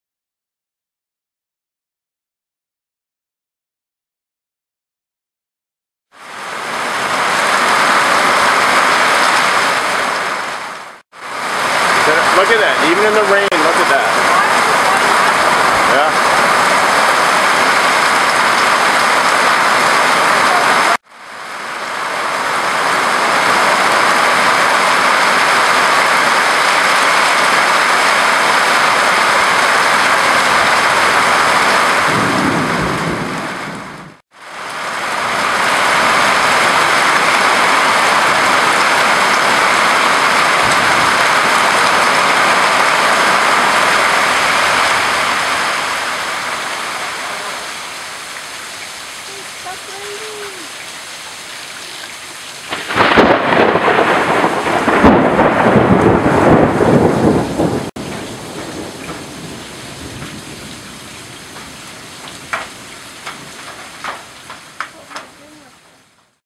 LighteningFire 9 6 12
This fire was started by a lightening hit during a heavy monsoon Sept. 6, 2012, Tucson, AZ. There were no injuries or property damages.